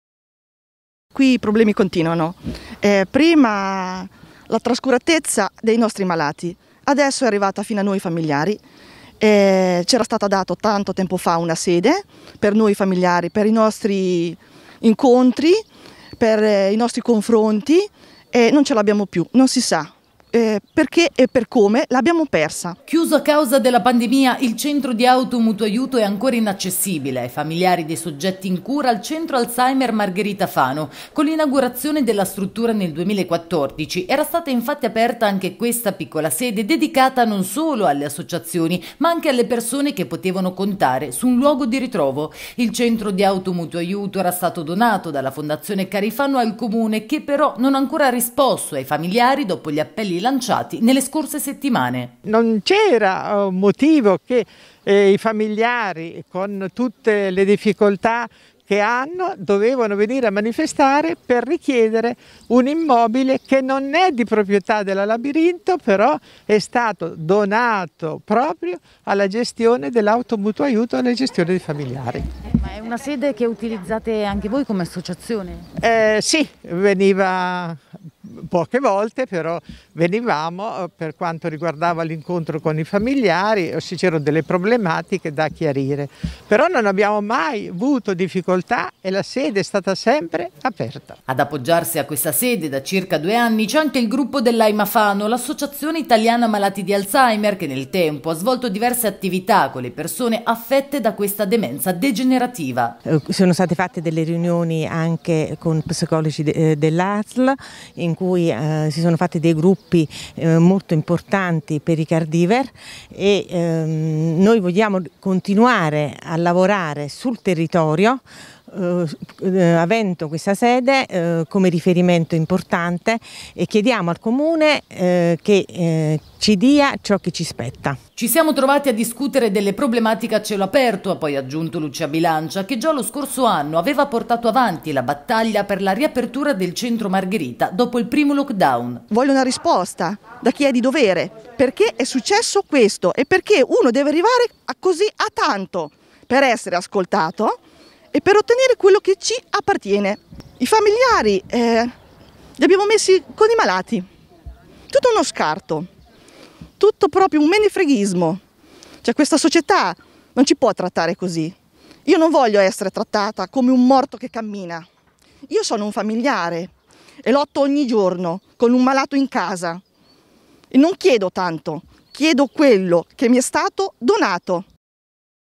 "Riaprite il centro di auto-mutuo aiuto"
Sit-in di protesta di fronte al Centro Alzheimer Margherita a Fano. Associazioni e familiari lanciano un appello al Comune per riaprire il Centro di chiuso da oltre un anno.